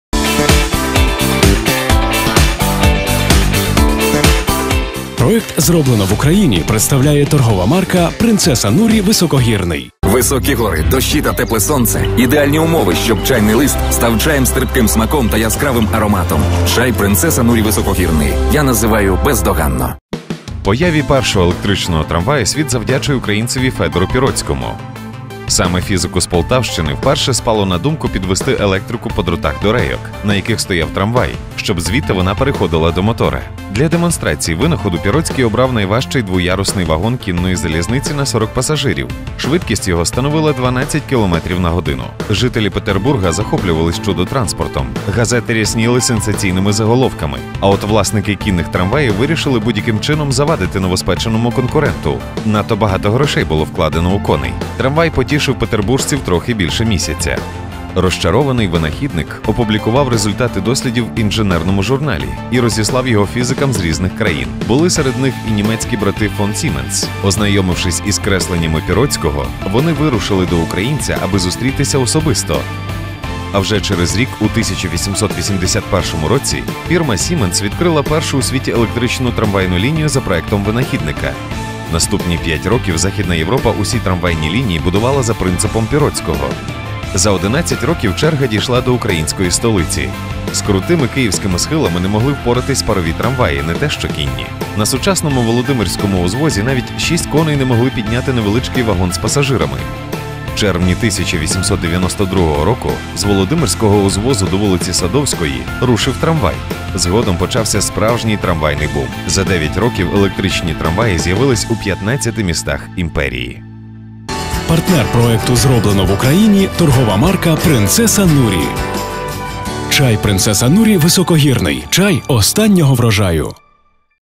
A program about Fyodor Pirotsky and the first world's electric tram on Ukranian TV
World's first electric tram was invented and made of two-decker horse tramway by Ukrainian-born Russian engineer Fyodor Pirotsky.